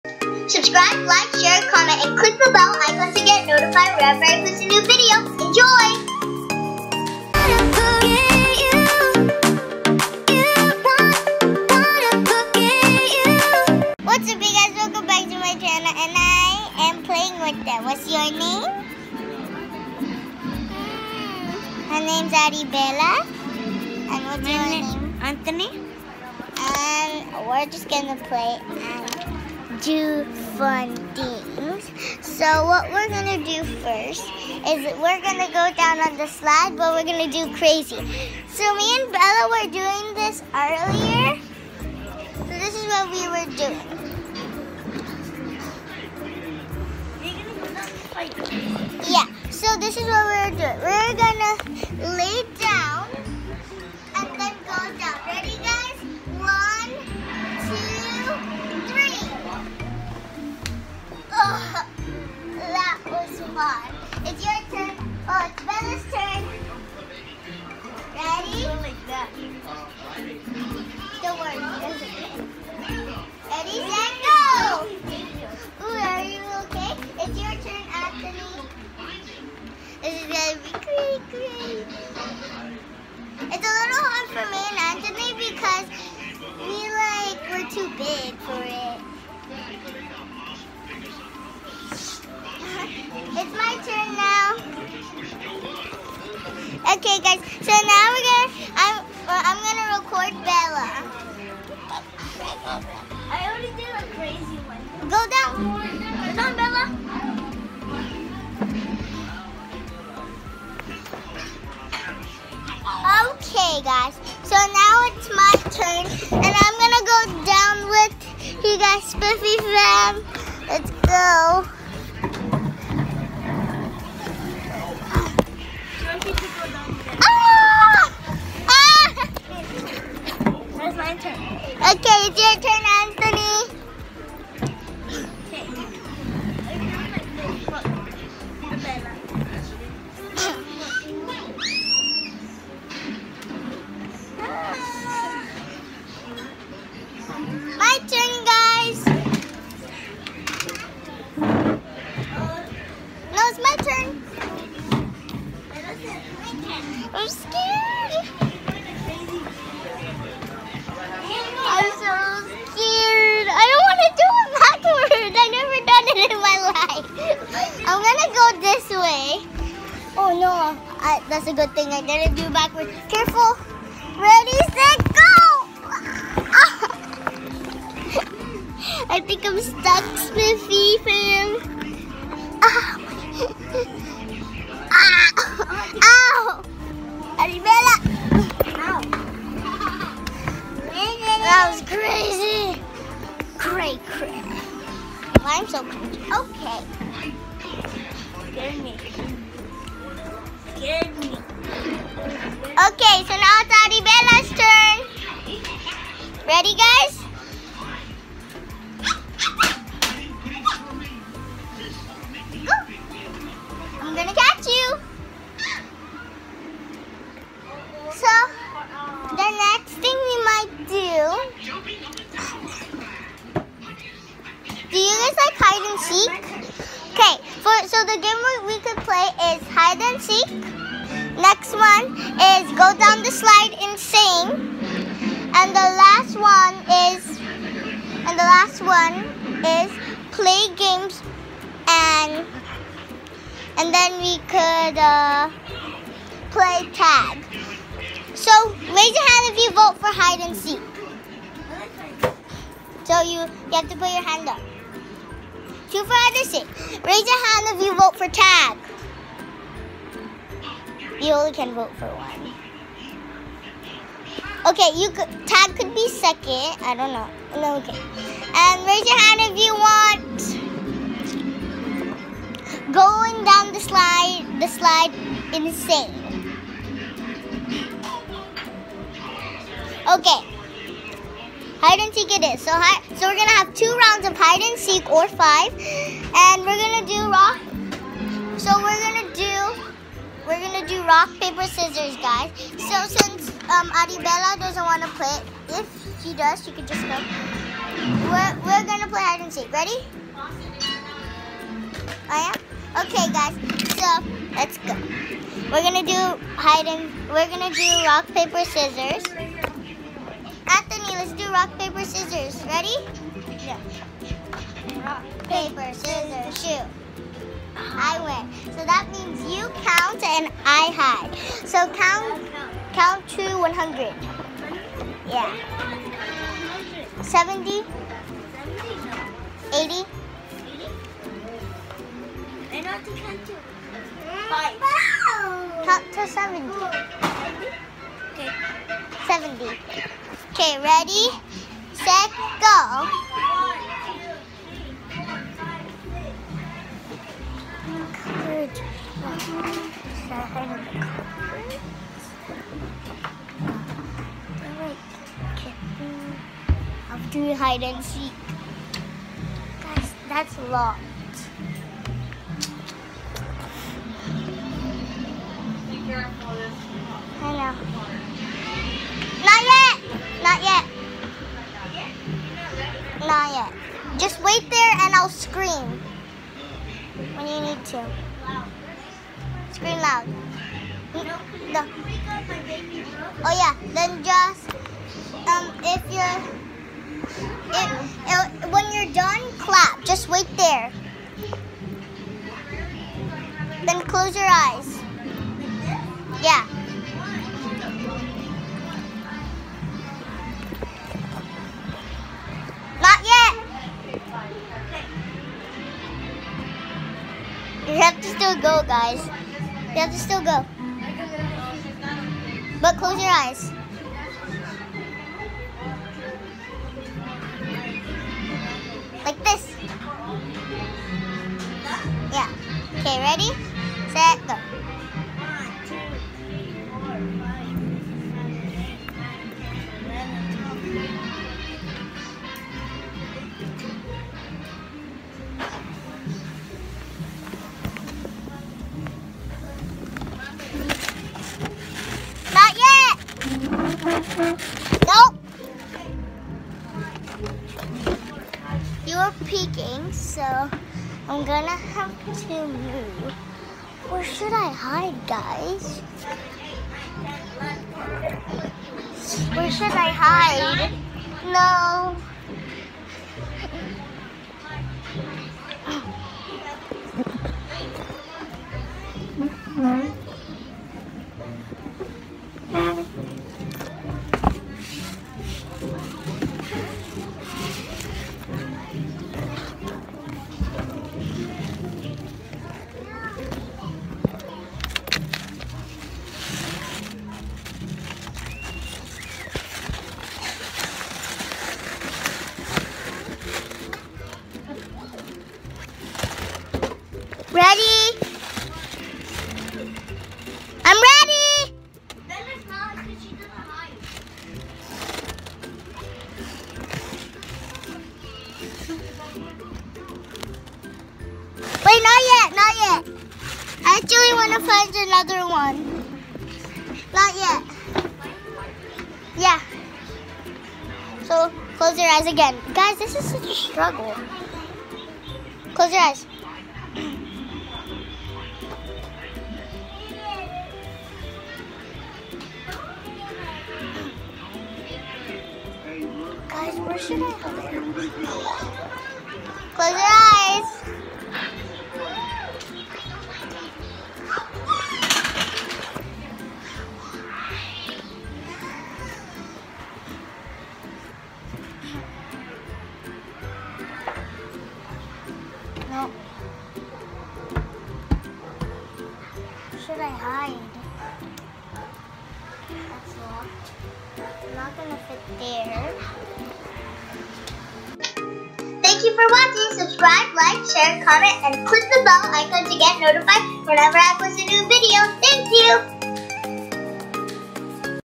SLIDING ON A TODDLER SLIDE (GONE WRONG) | Tiffy Spiffy LA
Hey Spiffy Fam, In this video my Mom & Dad came to visit their friends that we haven't seen in a long time and I played with their daughter. It was so much fun. I couldn't vlog as much because I left my camera at home and I was using my mom's phone but she ran out of memory that's why the video got cut off.